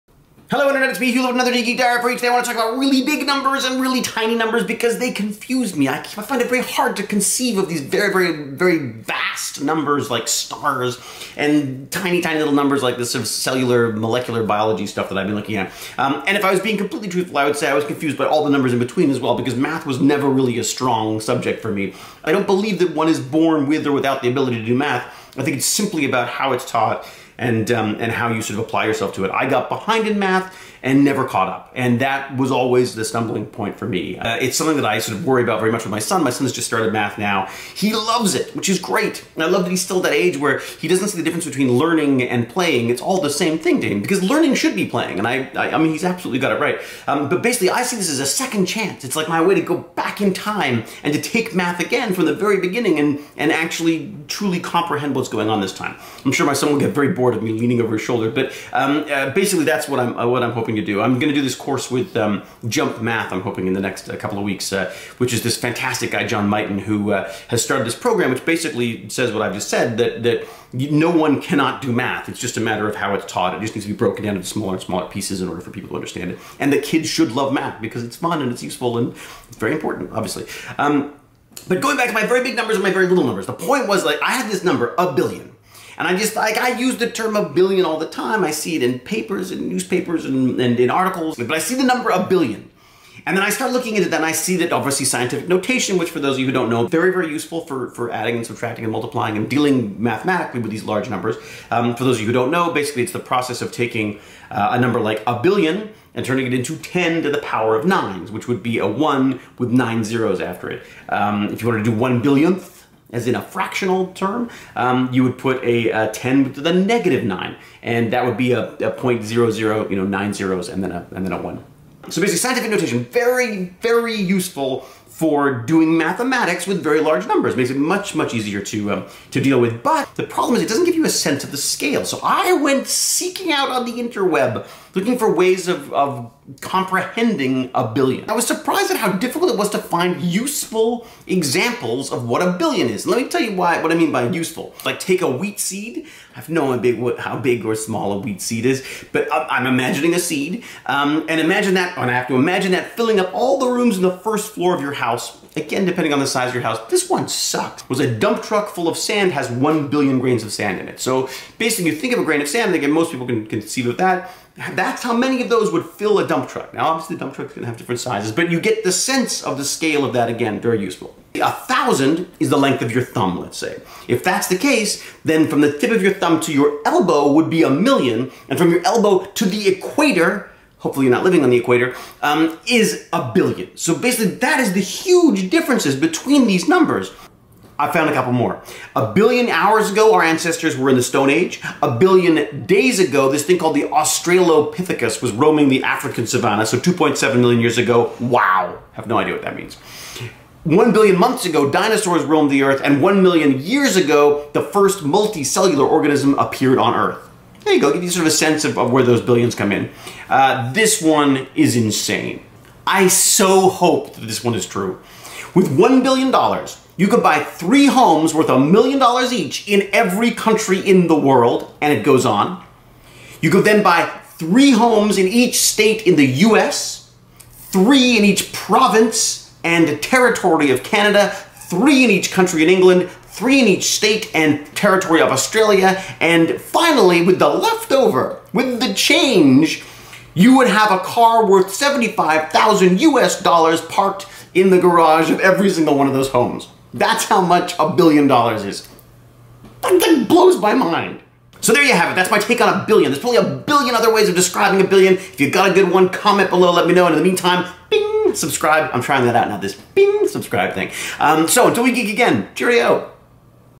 Hewletts Billions
I have a very hard time imagining very large and very small numbers.  They just don't mean anything to me.  Part of the problem I have is the fact that I am terrible at math. It was a subject that I hated at school...which I also hated. I ended up getting behind and that is the kiss of death when it comes to math.  I'm hoping to take a class or two of this Jump Math program that I'm intrigued by.
The program was actually created by this amazing guy here in Toronto, Canadian playwright John Mighton.  He wrote this amazing play I was in years ago, called "A Short History of Night".  I dream of one day making it into a feature film of some sort.  Amazing story of Kepler and his quest to unravel the mystery of the elliptical orbits of Mars. But that's not why you're here...you want to hear about Billions!